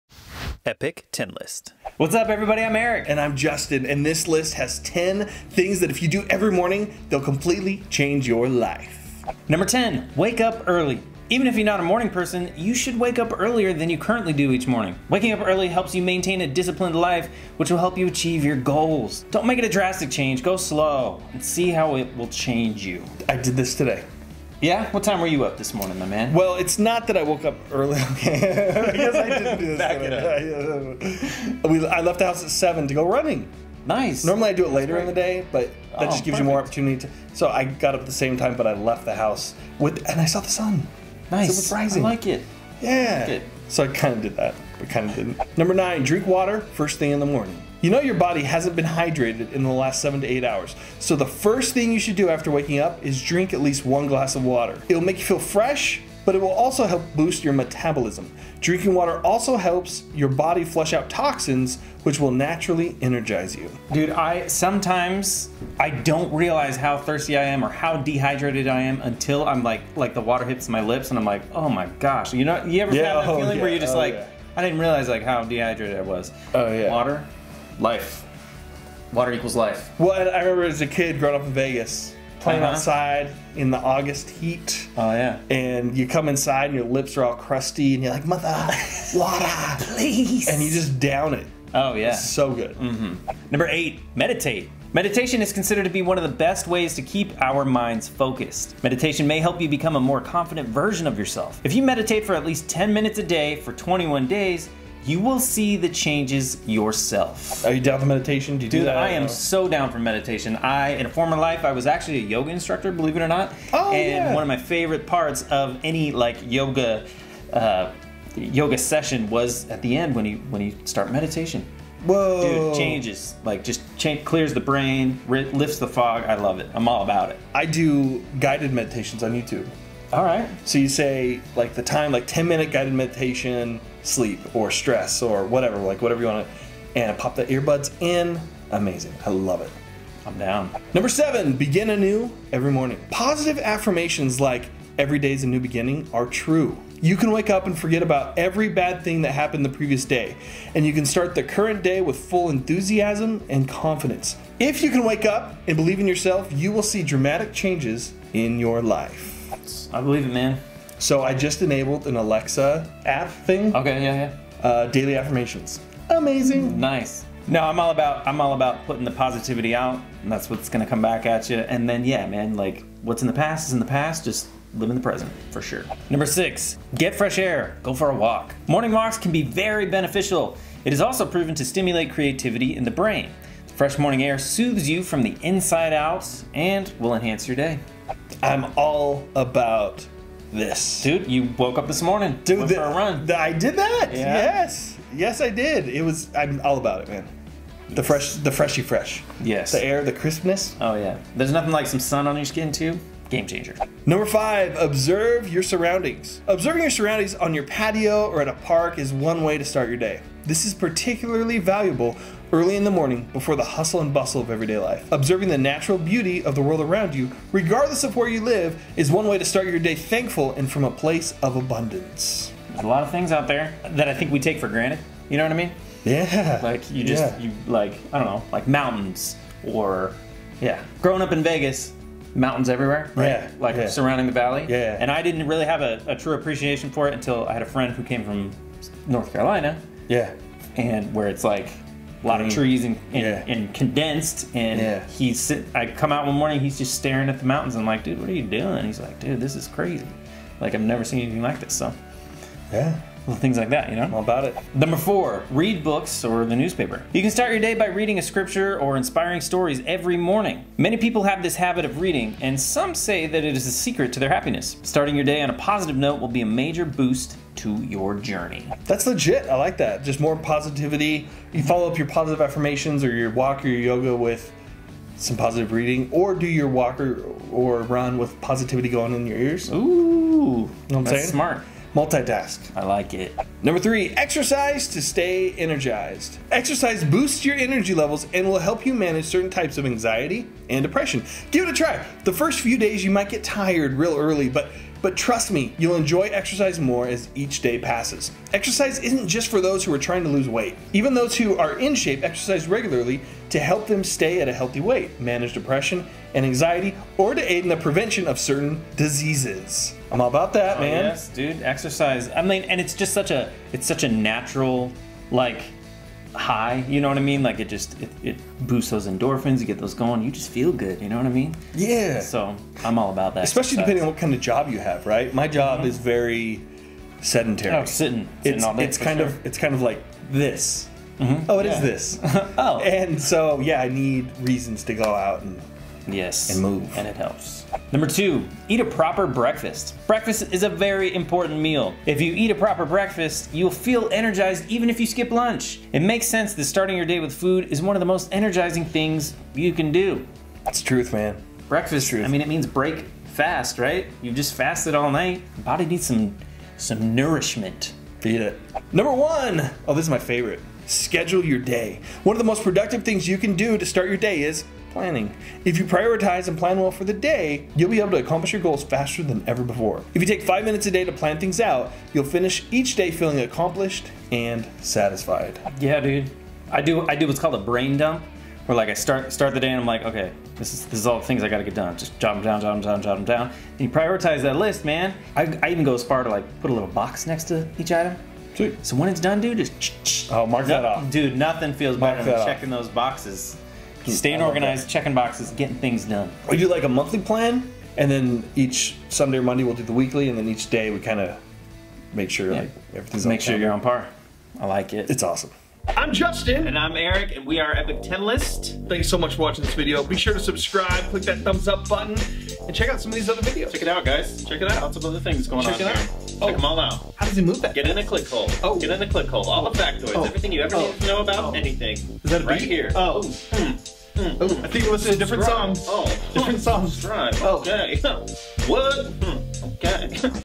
10 Morning Routines to Change Your Life
Wanna change your life?  Try these morning routines and your life is bound to change!